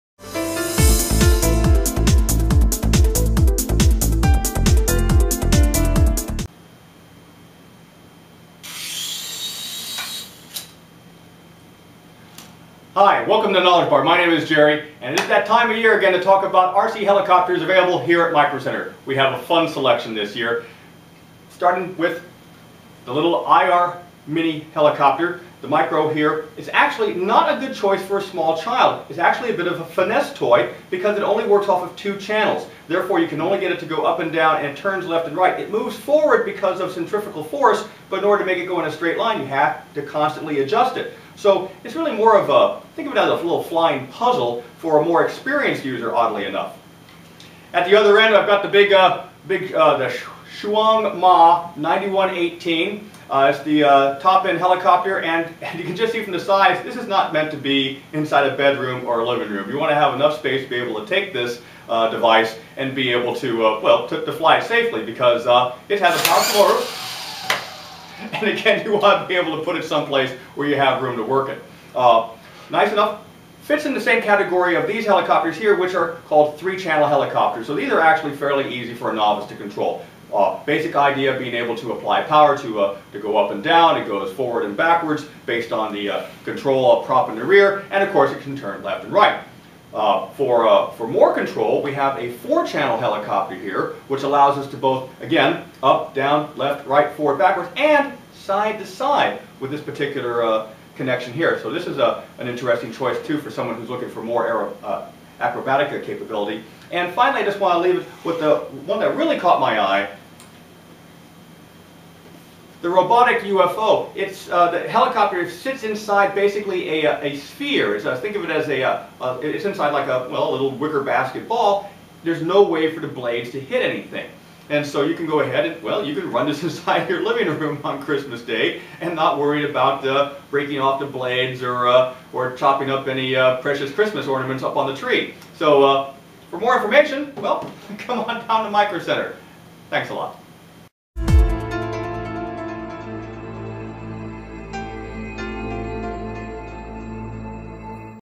Product: Overview of different RC Helicopters
Micro Center knowledge expert gives you a brief overview of different Remote Control Helicopters. Which one is best for you?